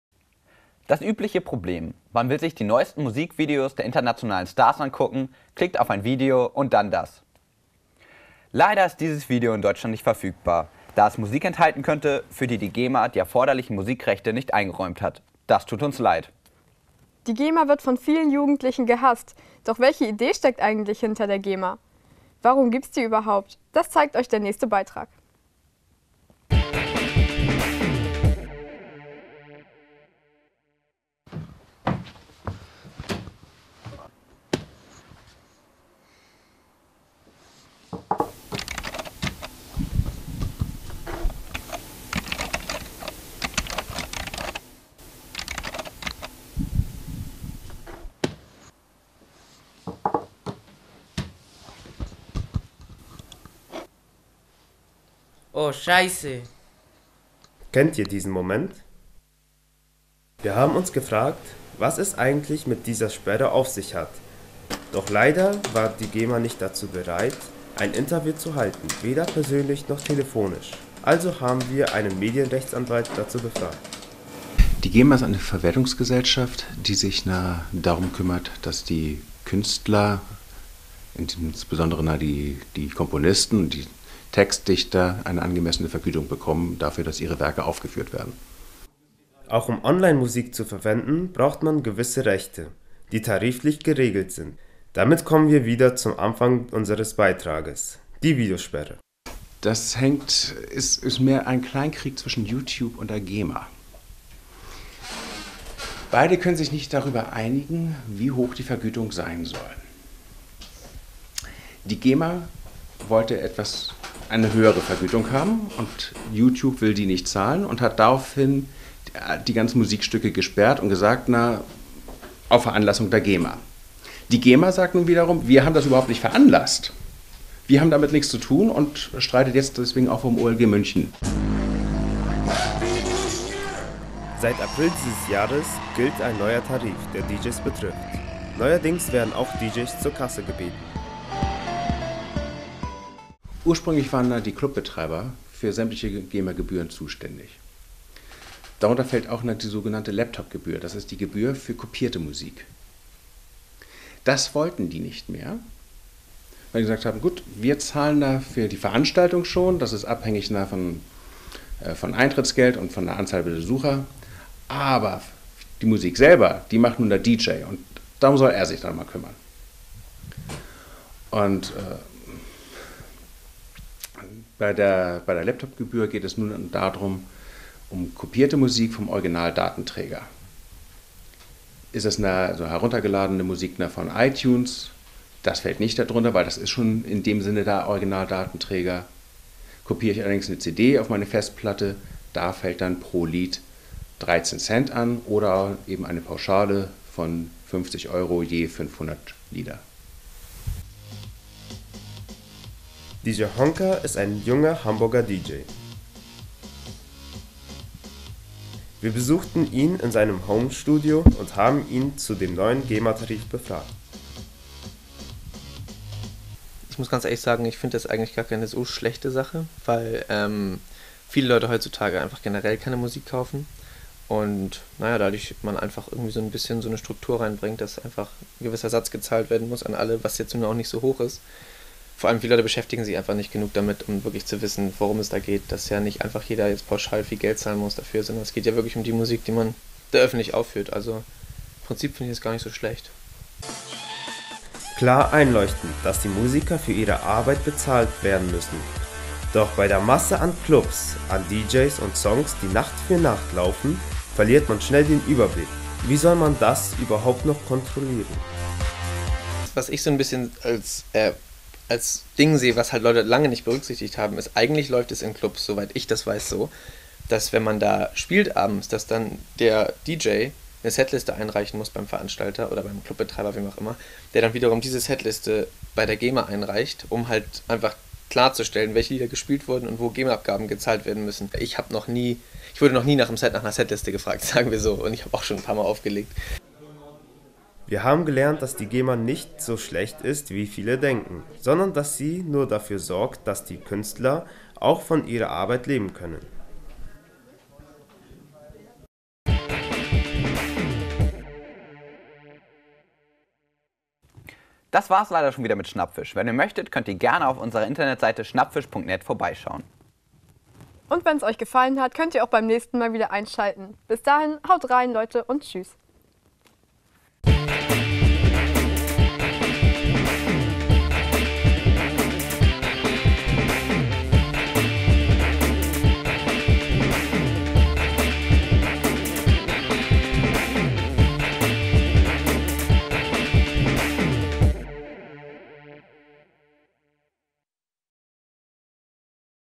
SchnappFisch - Gyula Trebitsch Schule - Die GEMA
Dieser Beitrag wurde von den Schüler_innen der neunten Klassen der Gyula Trebitsch Schule Tonndorf produziert.
Den meisten Jugendlichen dürfte die GEMA bekannt sein - aber was wissen wir eigentlich wirklich über sie?